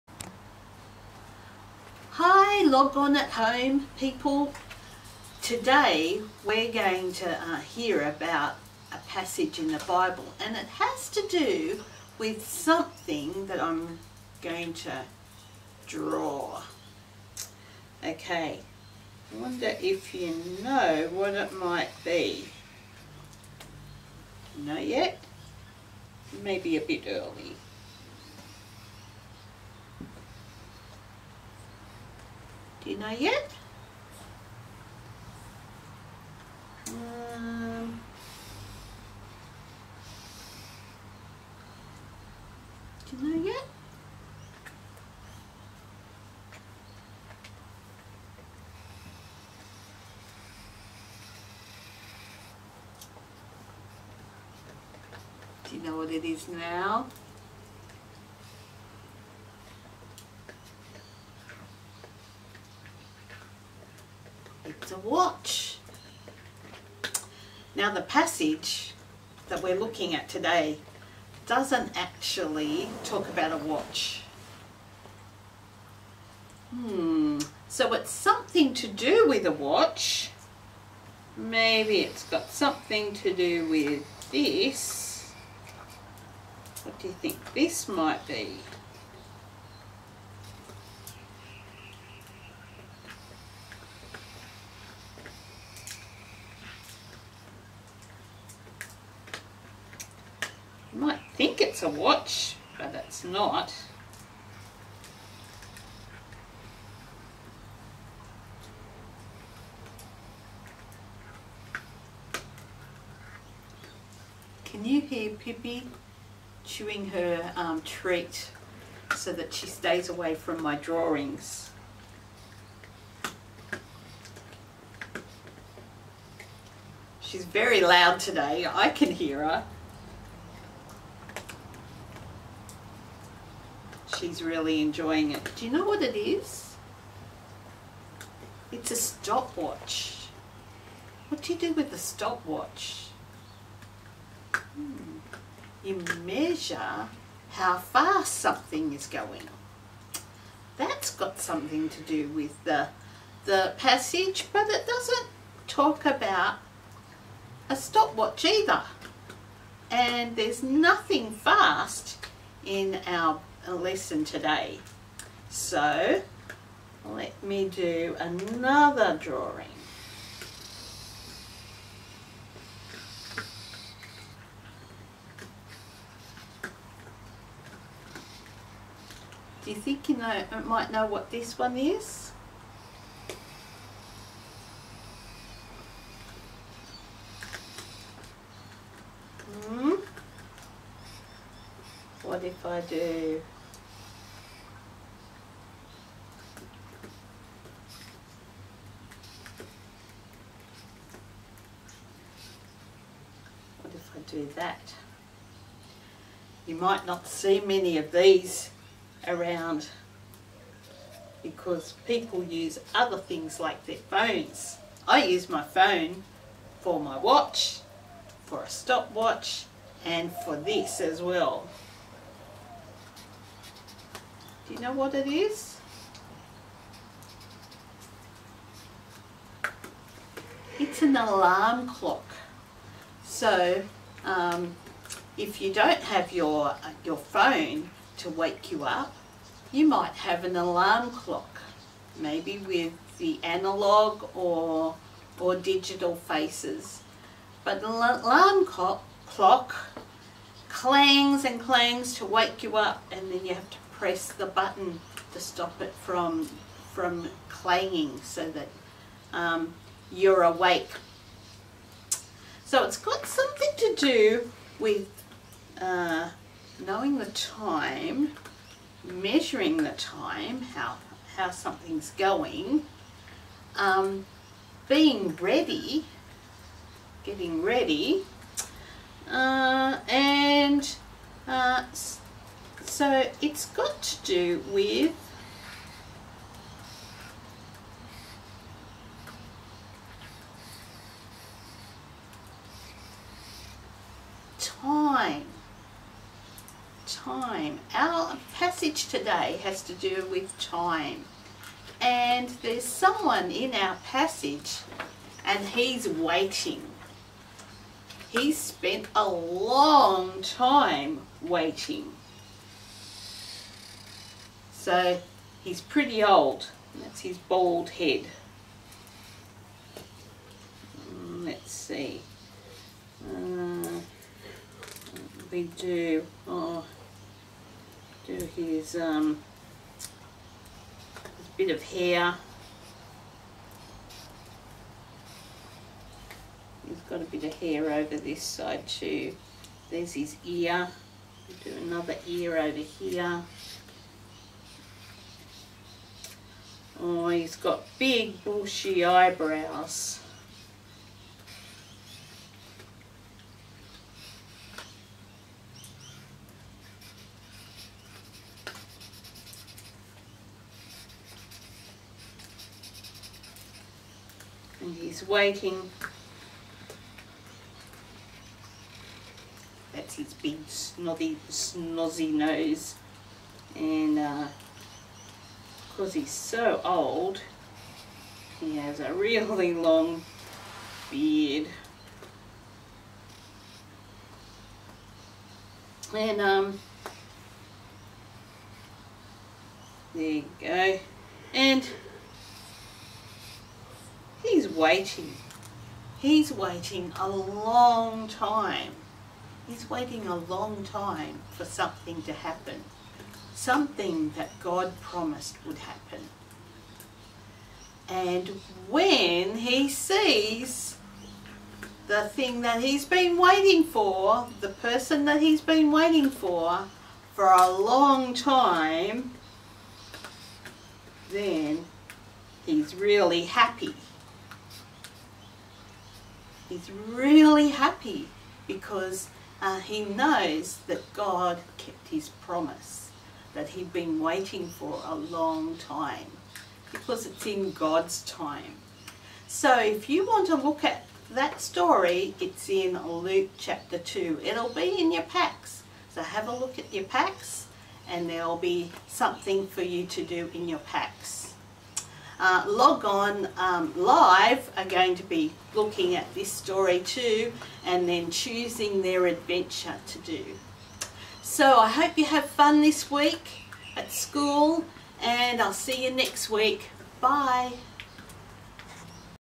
Family Spot - worth the wait
We are continuing in our series in Luke.  Finding out more facts that Dr Luke has written down.  The first part of Luke chapter 2 is very familiar we heard it at Christmas - Log On are looking at the next section after the Shepherds.